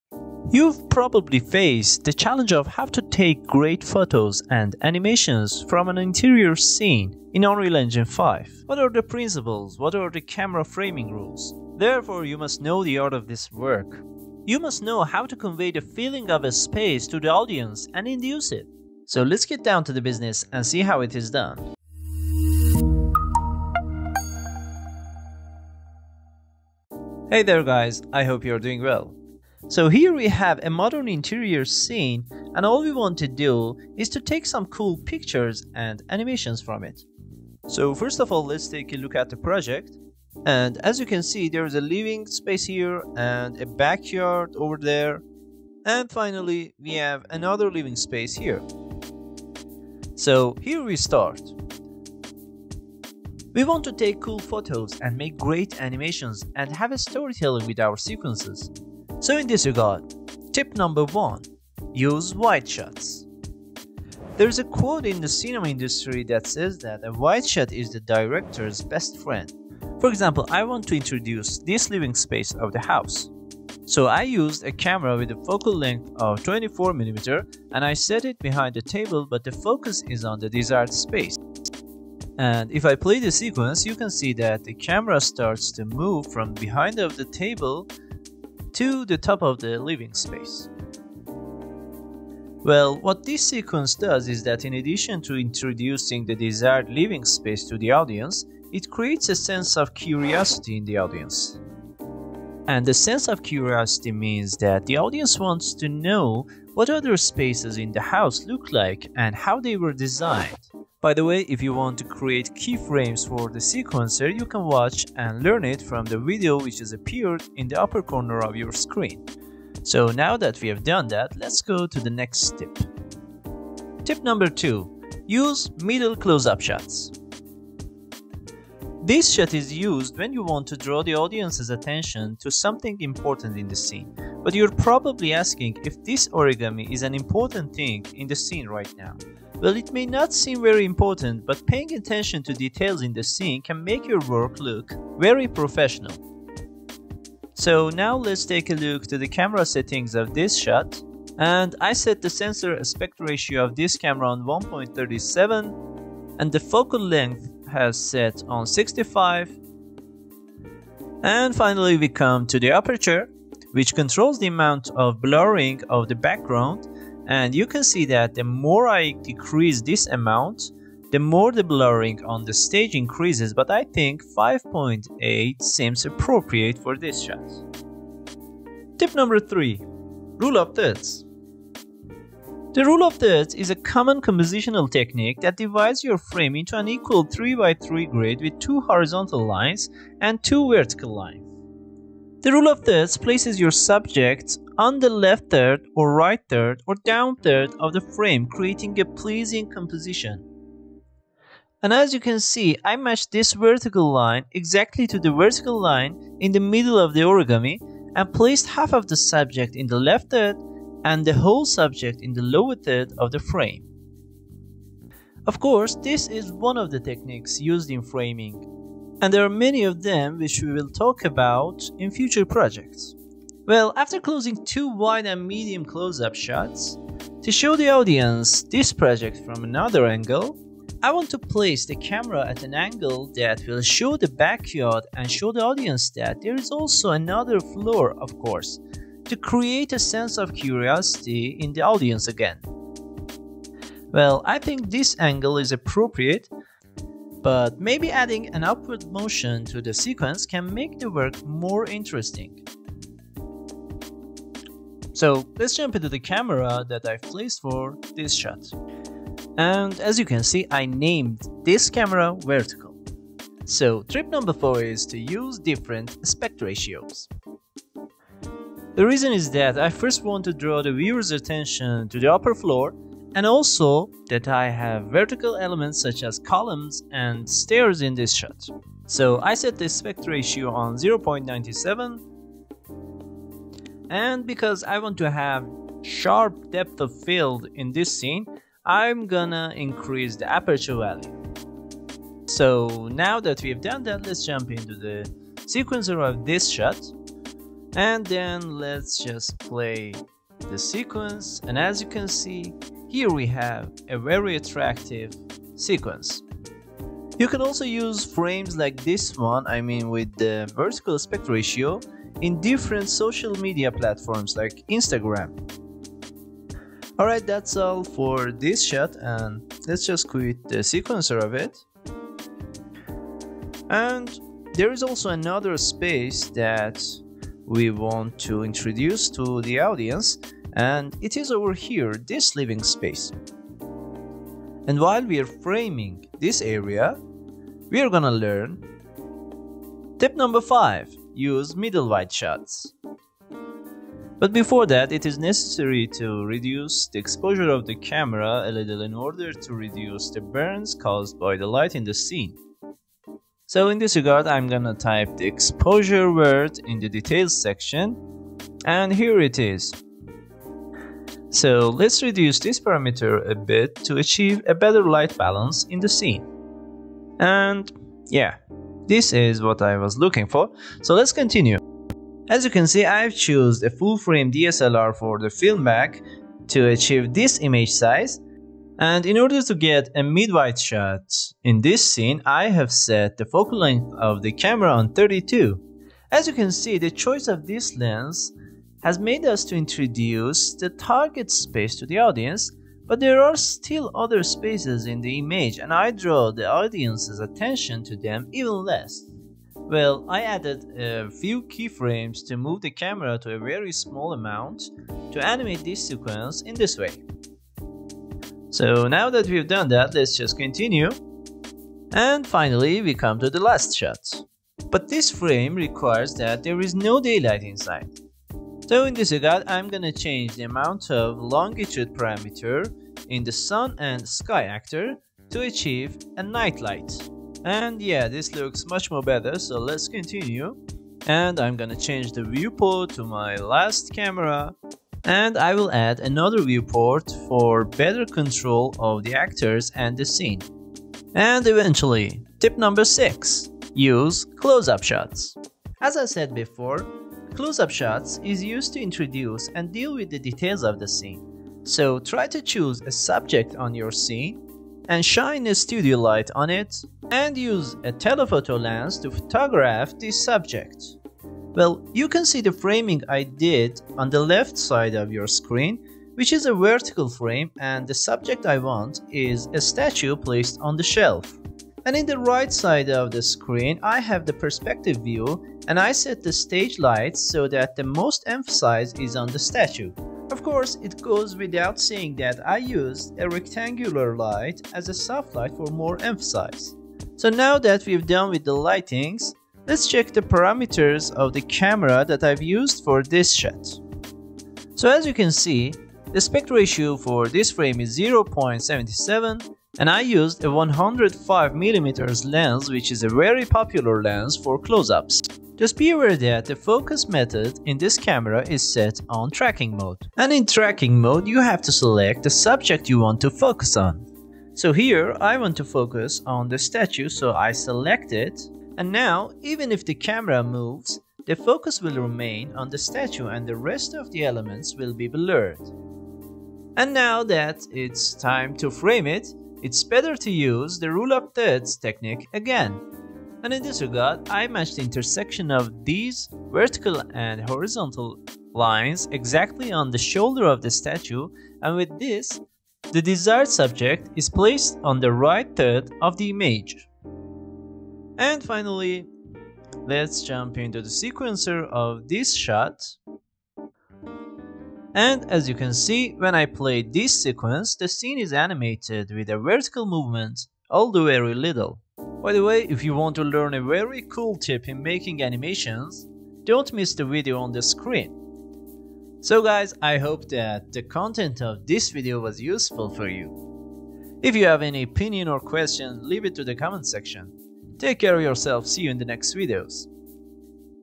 6 TIPS to take GREAT PHOTOS in Unreal Engine 5
As the title suggests, "6 TIPS to take great photos in Unreal Engine 5" gives you a very good approach to framing and rendering animations in Unreal Engine 5.

chapters
00:00 intro
01:00 Tip 1: Wide Shots
02:05 Tip 2: Mid-Closeup
03:00 Tip 3: Rule of Thirds
04:26 Tip 4: Aspect Ratio
05:42 Tip 5: Mid-Wide SHot
07:23 Tip 6: Closeup